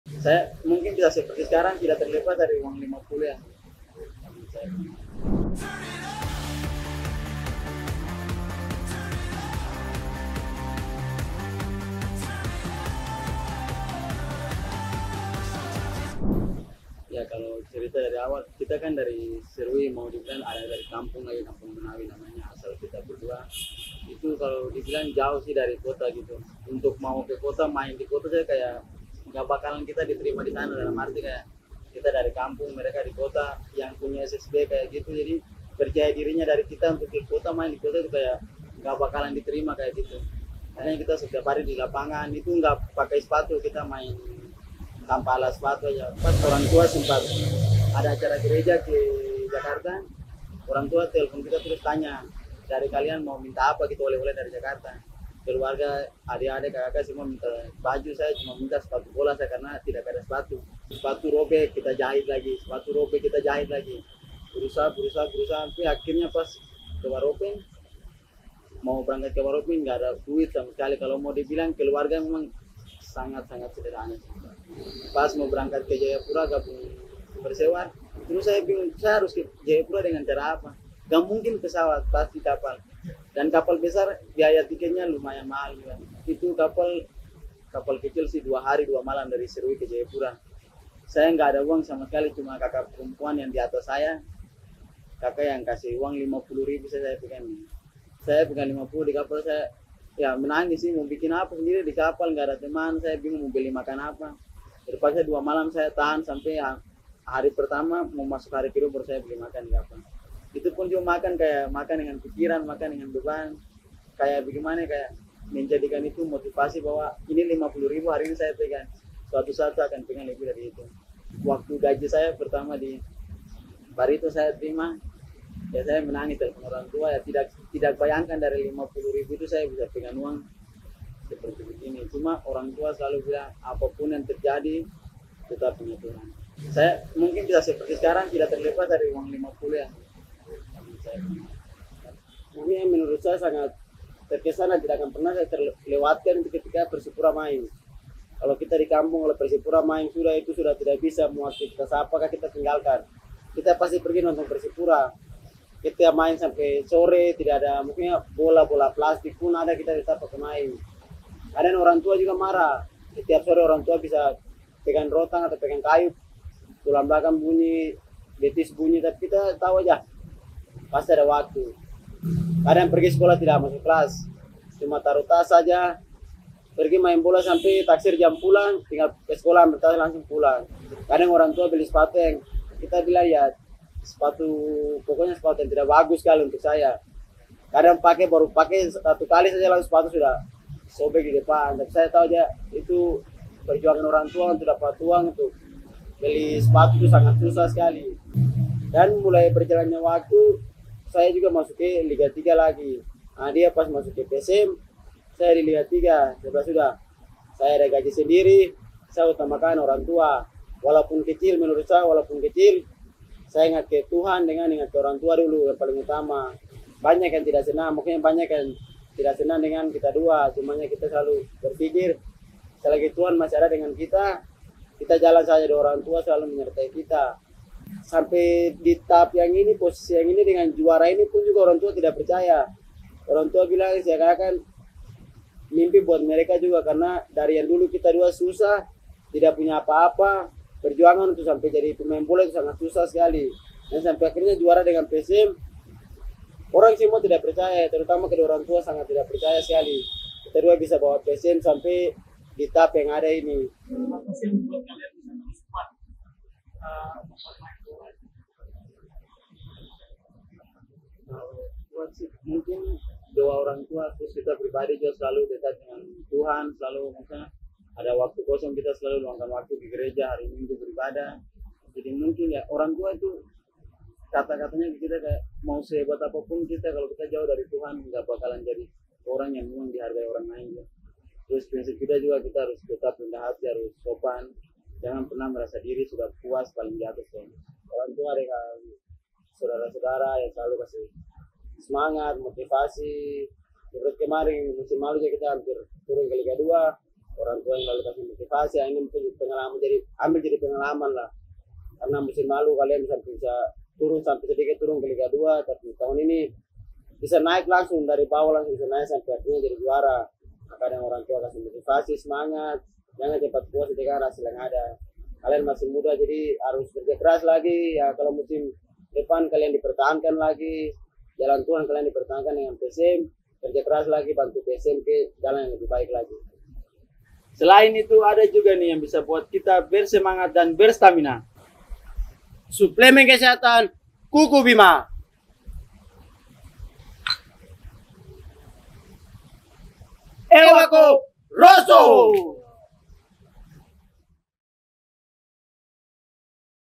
(EXCLUSIVE INTERVIEW) YAKOB SAYURI YANCE SAYURI
(Exclusive Interview) Kisah perjuangan dari 2 bersaudara yang selalu bersama yaitu Yakob Sayuri dan Yance Sayuri.

Semangat Rosa! dari dua bersaudara yang bercerita bagaimana strugle nya hidup mereka, dari anak kampung yang tidak punya apa apa sampai menjadi seorang yang di elu elu kan namanya.